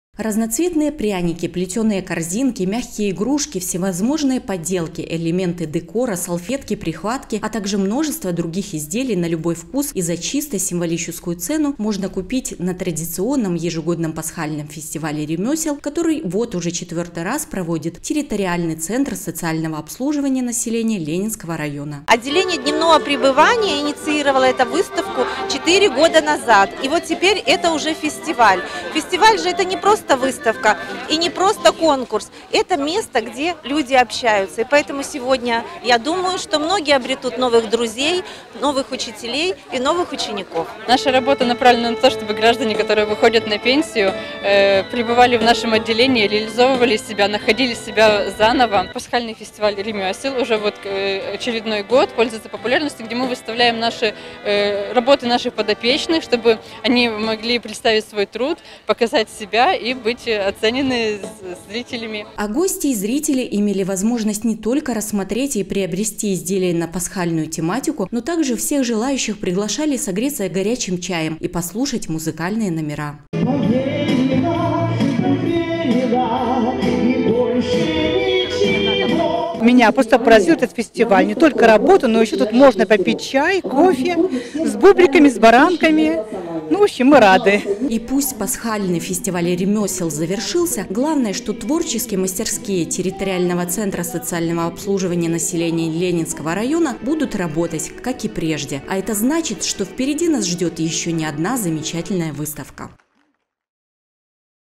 2021-04-28 г. Брест. Пасхальный фестиваль в ТЦСОН Ленинского района. Новости на Буг-ТВ.
Совсем скоро наступит светлый праздник Пасхи – в этом году он приходится на 2 мая. Каждый готовится к этому дню по-разному –одни соблюдают пост, а другие делают обязательную генеральную уборку, покупают краску для яиц и готовят подарки для родных и близких. А вот Территориальный центр социального обслуживания населения Ленинского района ежегодно в преддверии праздника проводит Пасхальный фестиваль ремёсел, на котором побывала и наша съёмочная группа.